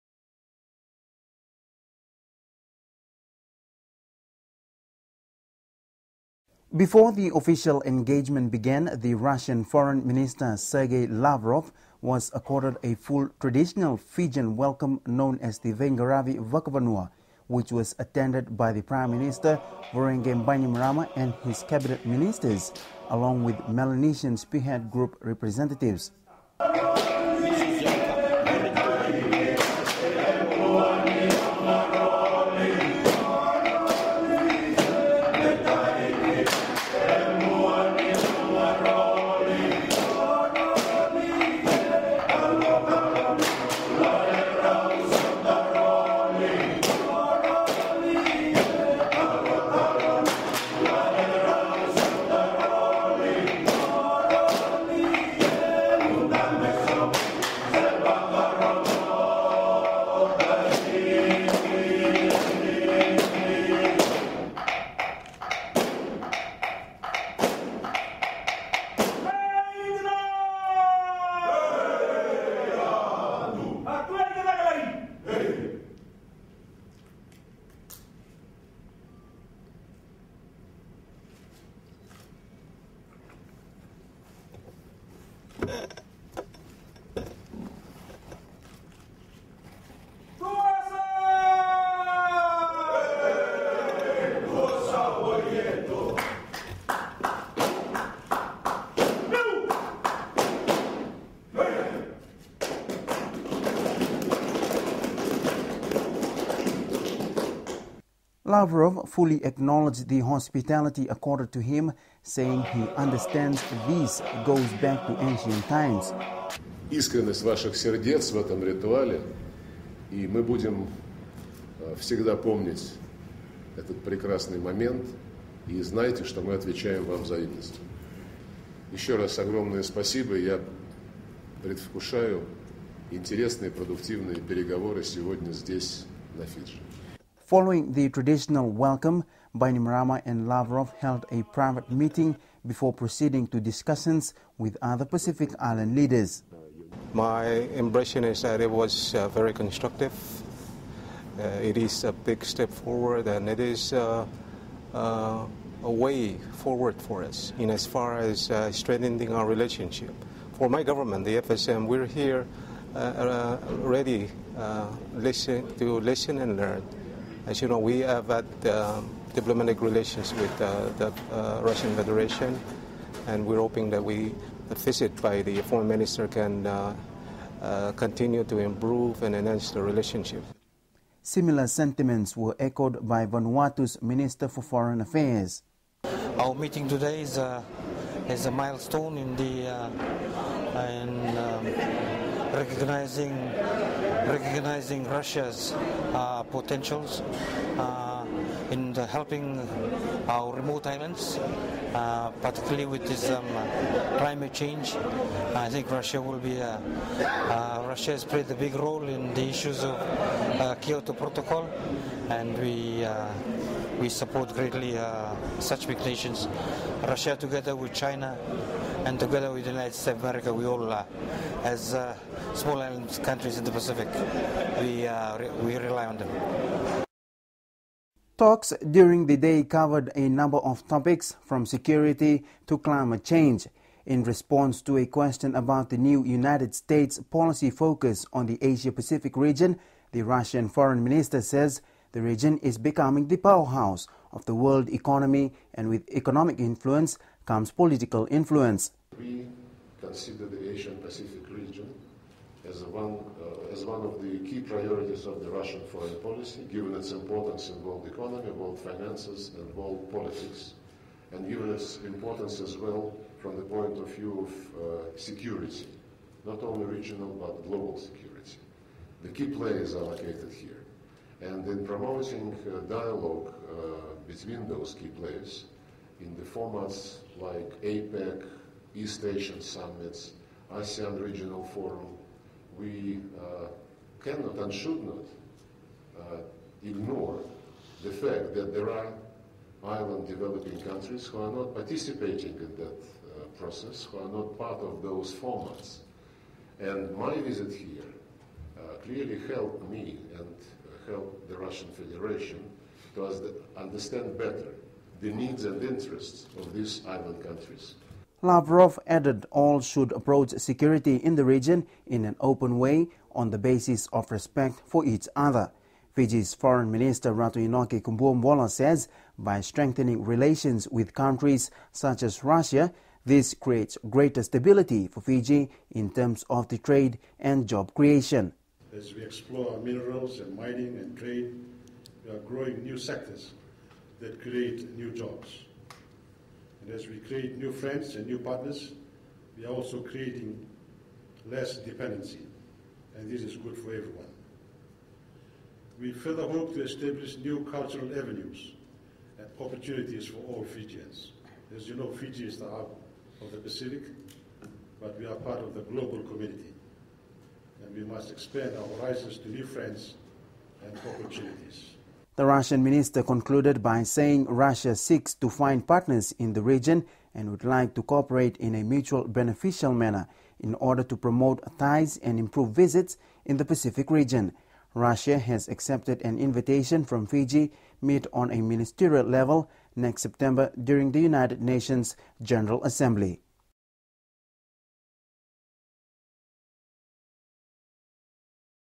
Russian Foreign Ministers Fiji Visit
4th February 2012: The Russian Federation's Minister of Foreign Affairs Sergey Lavrov arrived in Fiji on Wednesday 1st February for his country's first-ever high-level visit. The Foreign Minister was greeted by a traditional Fijian welcoming ceremony, attended by Fijian Prime Minister Josaia Voreqe Bainimarama.

The Fijian Prime Minister and Minister Lavrov then commenced individual discussions. Minister Lavrov met individually with the Fijian Prime Minister and then with the Fijian Minister of Foreign Affairs & International Cooperation, Ratu Inoke Kuabuabola.

A number of topics were discussed, including ways of promoting economic cooperation between the two nations. Pursuing educational assistance and cooperation between Russia and Fiji was one of the issues discussed.

"We also talked about more specific cooperation on the UN agenda and we agreed that our ambassadors to the United Nations should meet on a regular basis in New York to discuss the current status of things and elaborate on approaches".

The meeting in Fiji, the first high-level visit from a Russian official to the country, afforded the Russian Federation the opportunity to strengthen ties with Fiji, as well as meet with numerous other foreign ministers from the Pacific Island Countries.

As Minister Kubuabola said during the press conference, "Fiji is the hub of the Pacific, but we are a part of the global community. We must expand our horizons to new friends and opportunities."

Conversations during the day covered a number of topics, from security to climate change. The economy however was the central issue of discussions. "We've got the common understanding that the priority of the day are the programs and projects that will drive real growth and make the economies of the [Pacific] region even more independent," said Minister Lavrov.

Minister Kubuabola concurred: "By diversifying our relations with such countries as Russia, we are creating greater stability for ourselves. We hope to establish new lines of trade that help diversify the types of job opportunities there are for Fijians."

The meeting resulted in a number of outcomes between Fiji and Russia, including an outline of areas of economic cooperation in which "concrete" next-step proposals would be created, an accord to establish visa-free travel to promote tourism between the countries, and a look at how Russia can support Fiji's contributions to peacekeeping missions around the world.

The Russian Minister concluded by saying, "We seek to find partners in the region, and we would really like to cooperate in a mutually beneficial manner in order to promote our ties and improve visitation in the region. And let me say that we are not making friends against anybody."

Russia has accepted an invitation from Fiji to again meet on the ministerial level next September during the United Nations General Assembly.